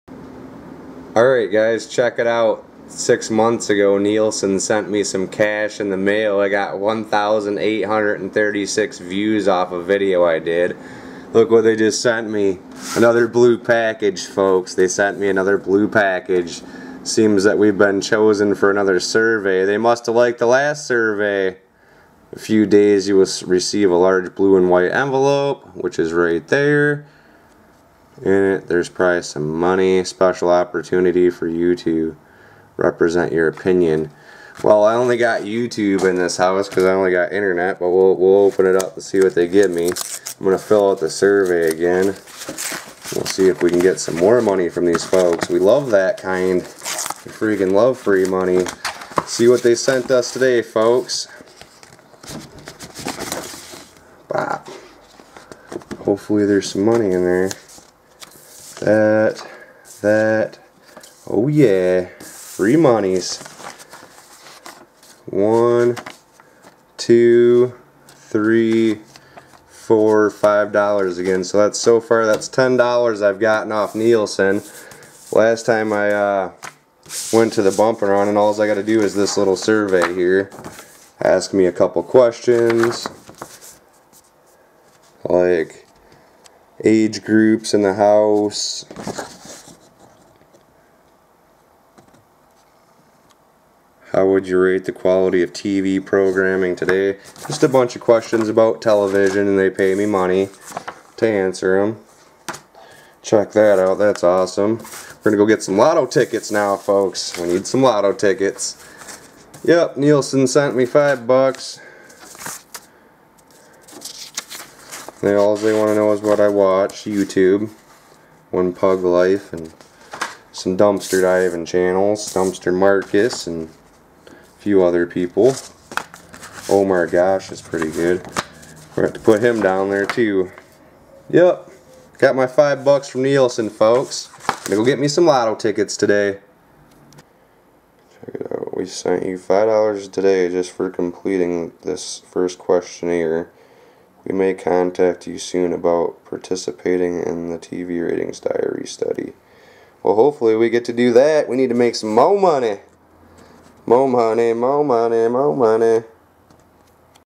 Earning Money Doing A Quick Survey!! Thank You Nielsen Television Ratings Company!!
Getting more cash in the Mail from Nielsen TV Ratings Company!! Cash in The Mail!! Cable Television Ratings Company!!